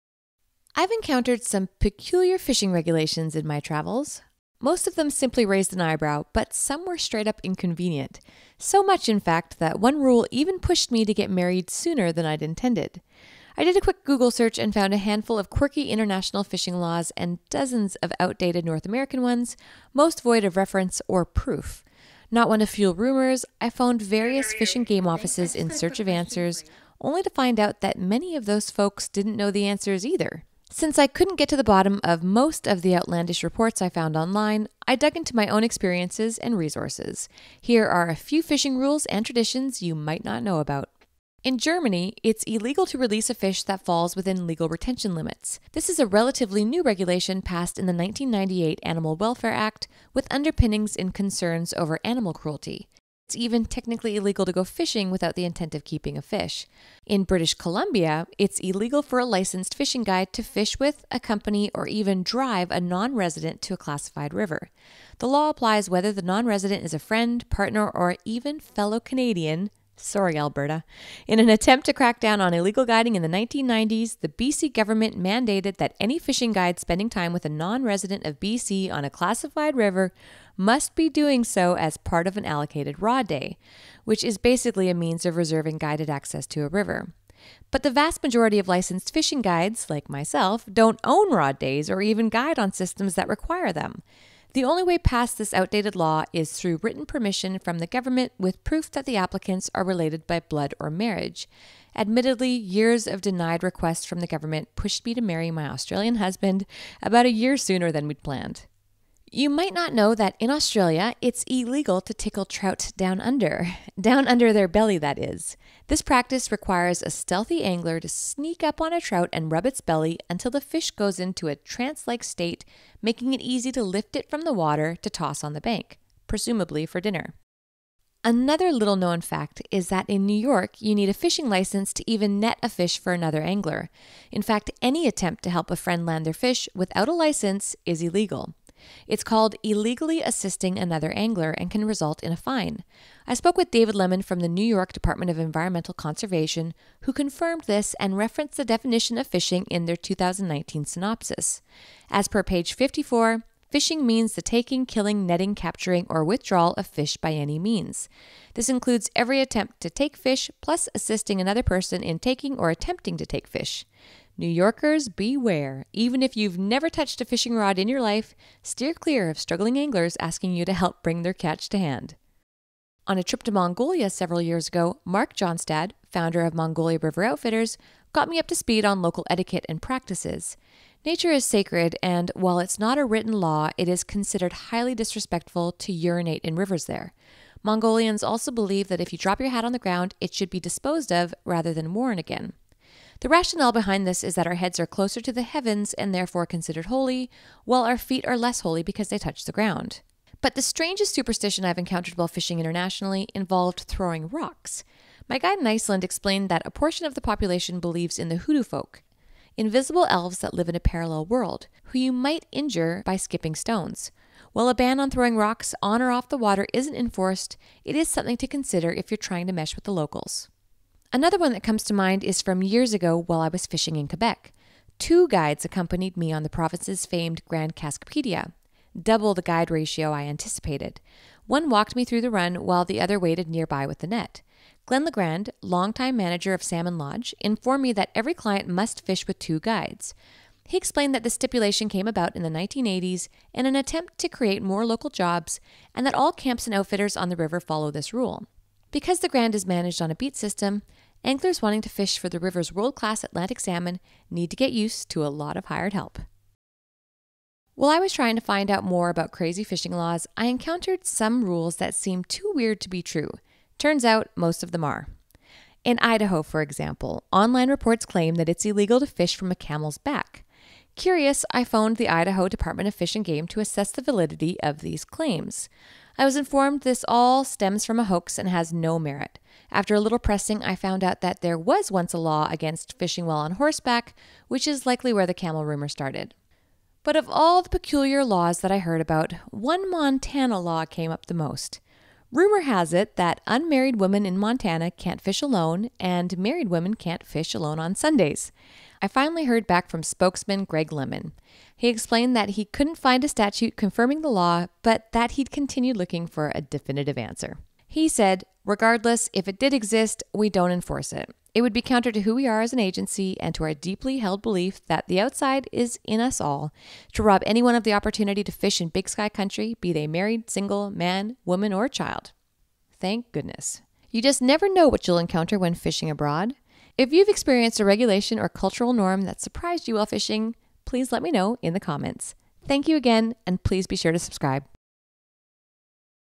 9 Strange Fishing Laws and Customs Around the World That You Might Not Know
I’ve run into some strange fishing regulations over the years. Most were amusing, but some were rather invasive. One outdated regulation even pushed me to get married.